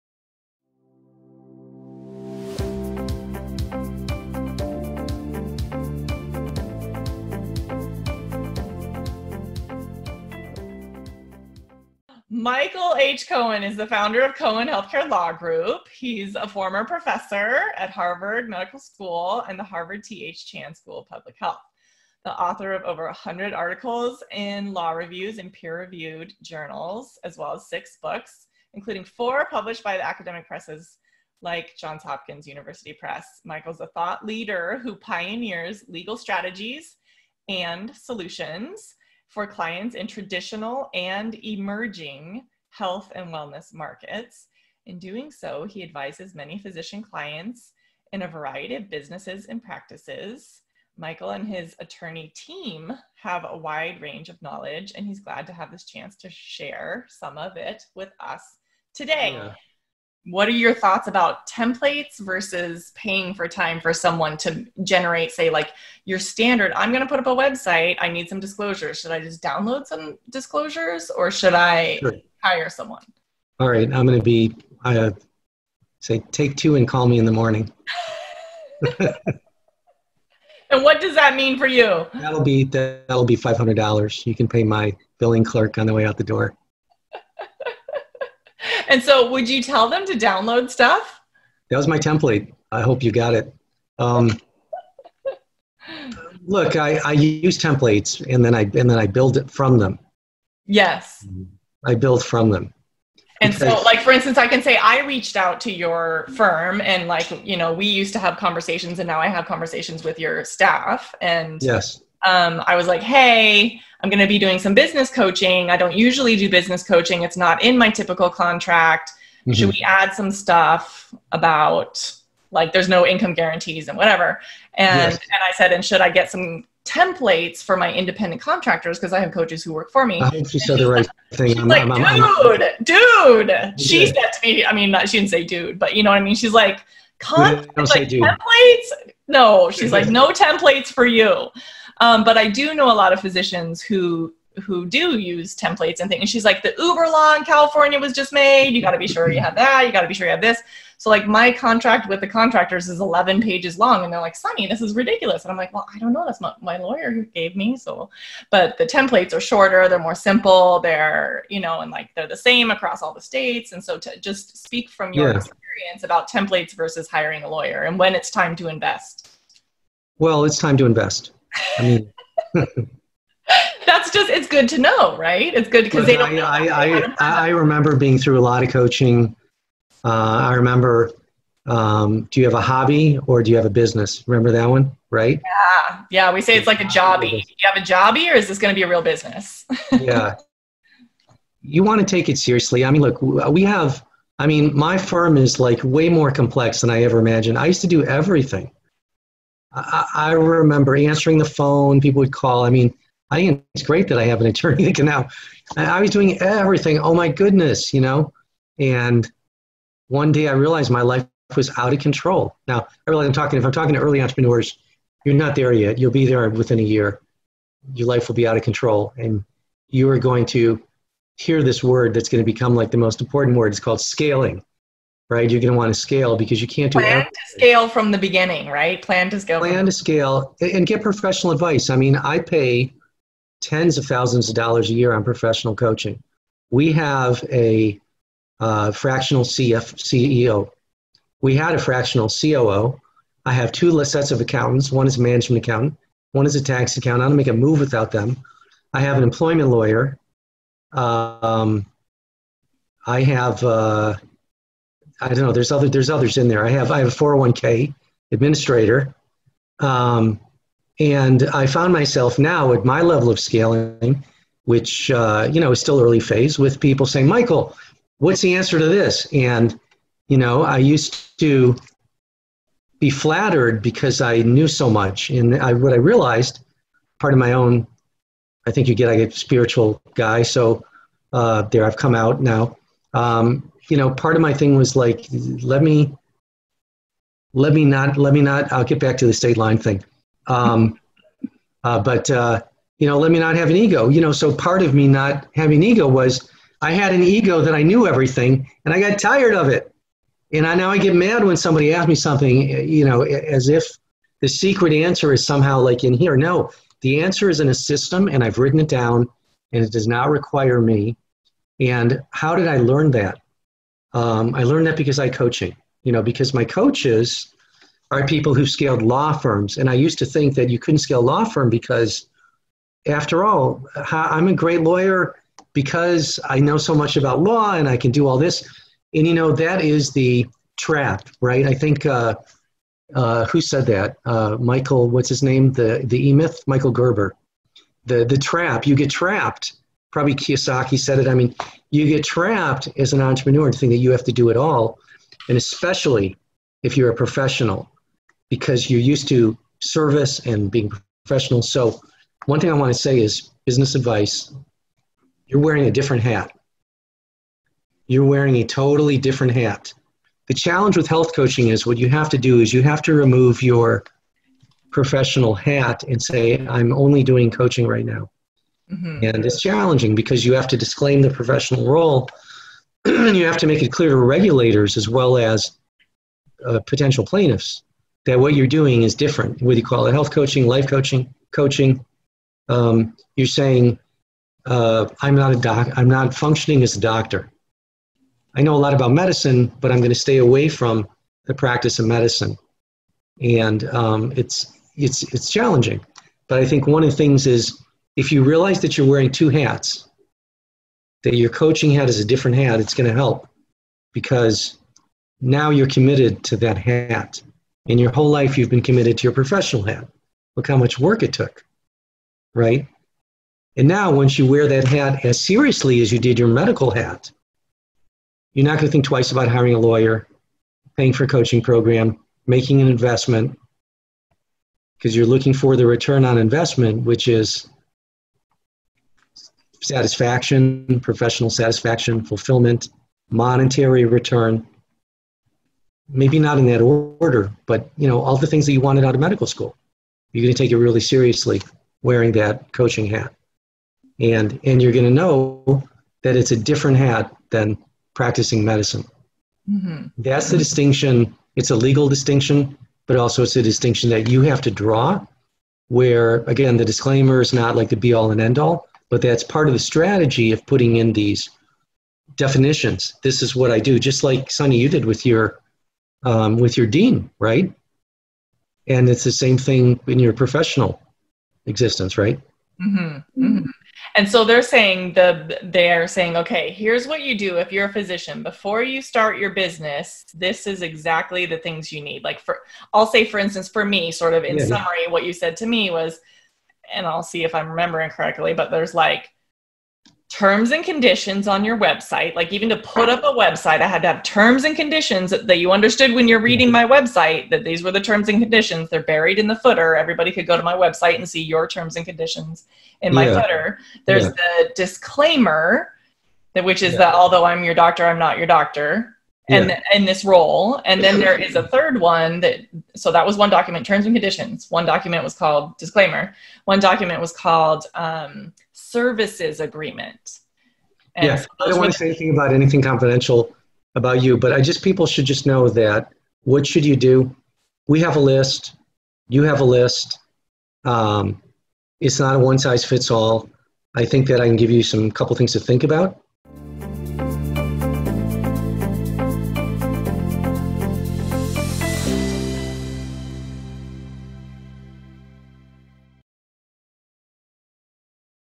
Expert Mentor Session Part 7: Should I Use Legal Document Templates?
This video is Part 7 of a 9-Part Series excerpted from an Expert Mentor Session held by Michael H. Cohen, JD and Dr. Sunny Smith, MD on July 3rd, 2020.